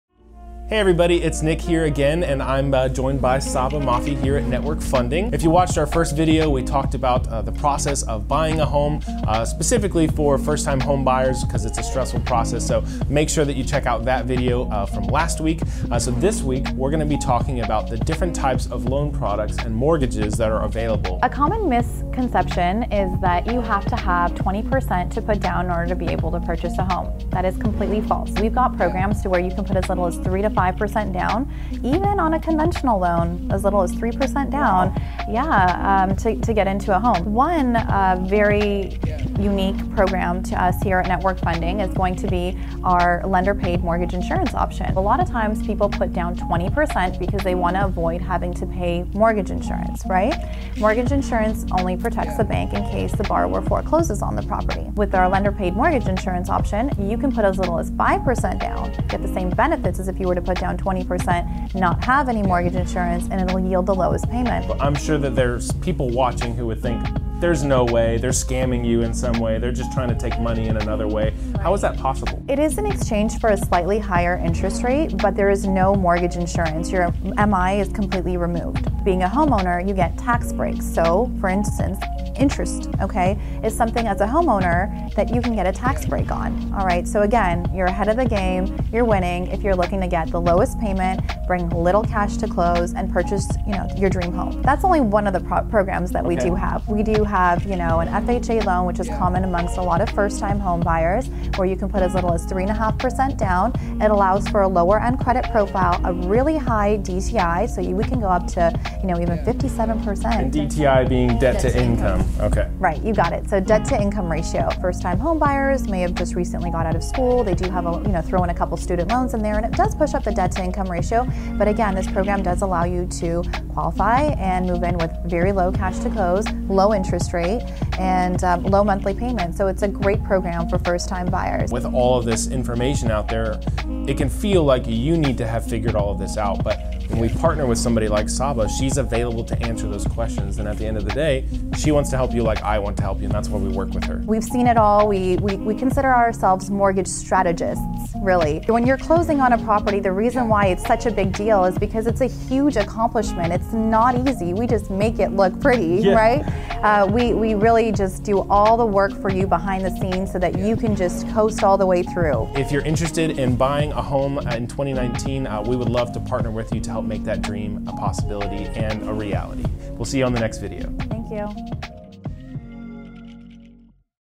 Do you need to put 20% down to buy a house?
In this video, we go over some common mortgage types, as well as some exclusive to Network Funding that help home-buyers purchase a home with as little as 3% down.